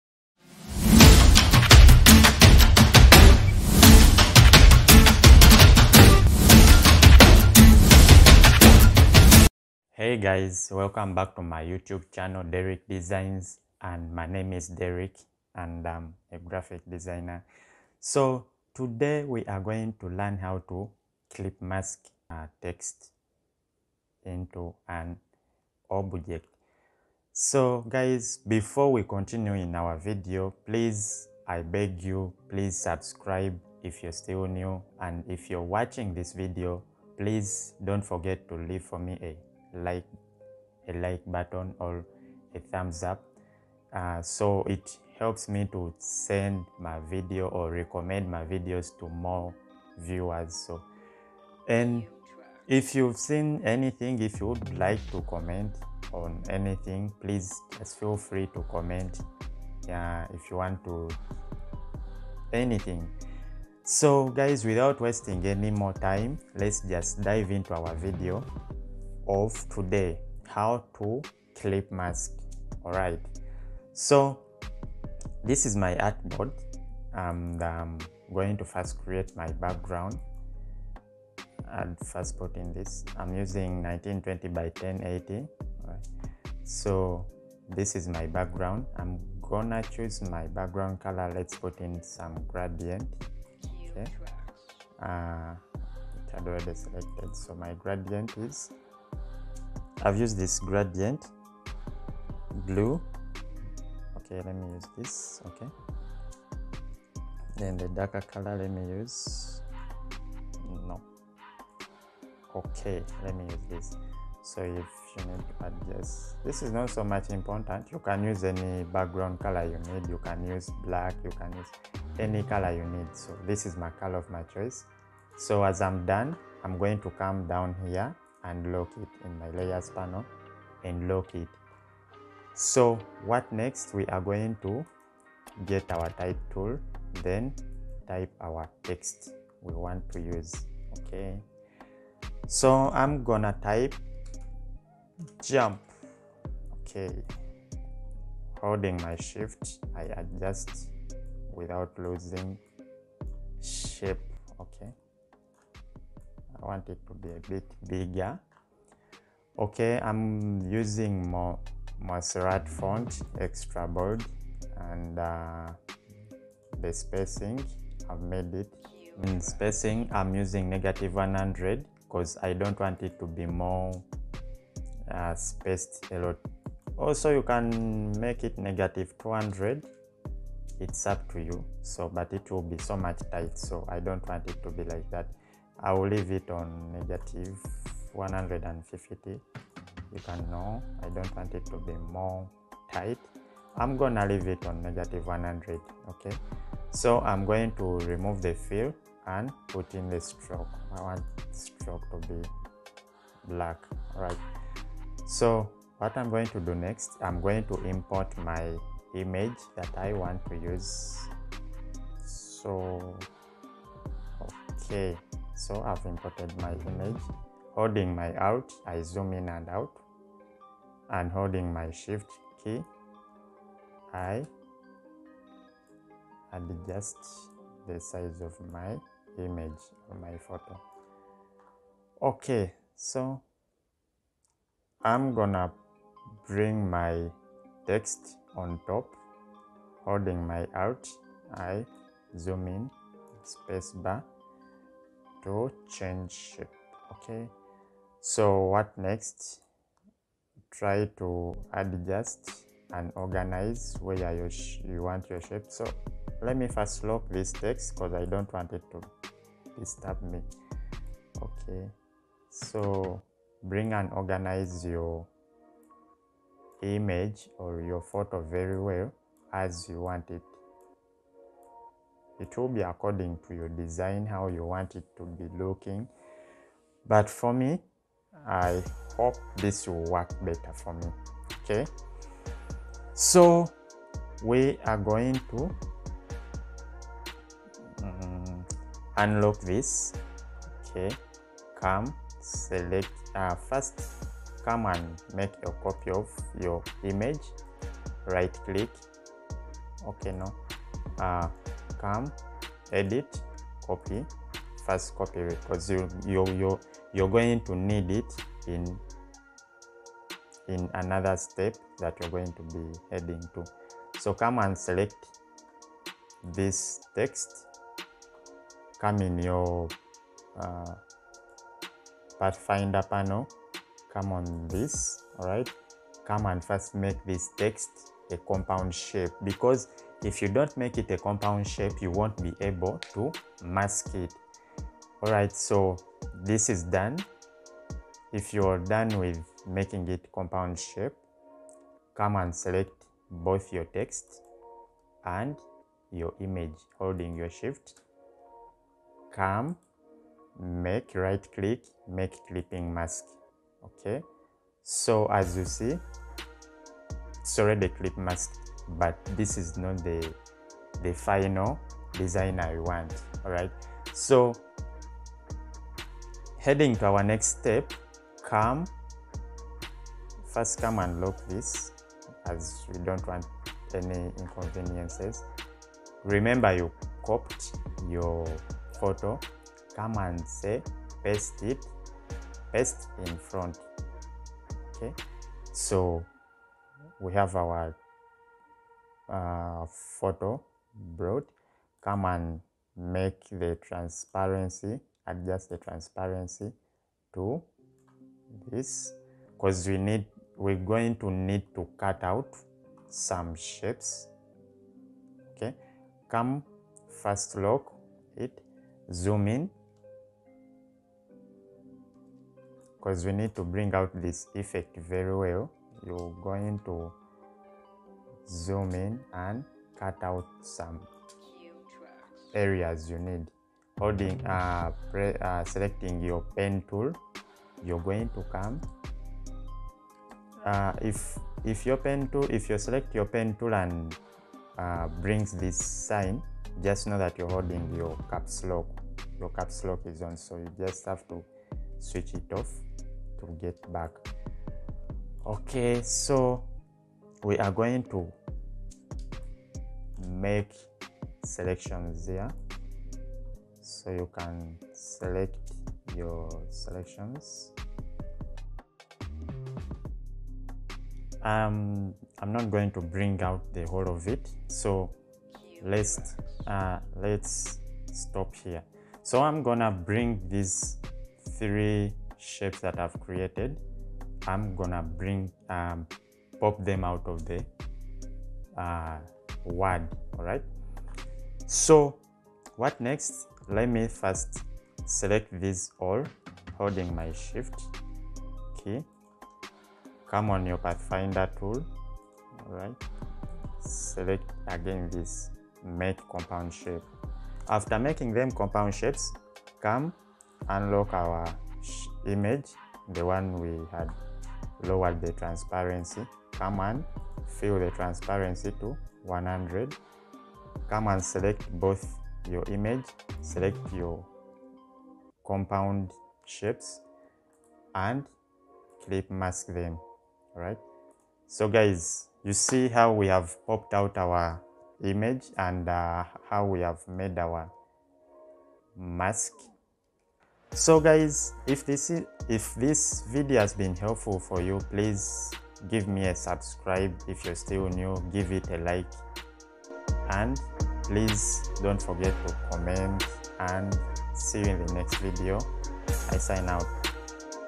How to create clipping masking Text In Adobe Illustrator | Adobe tutorials |BEGINNER
In this video you're going to learn how to clip mask photos in a text .

Software: Adobe illustrator 2021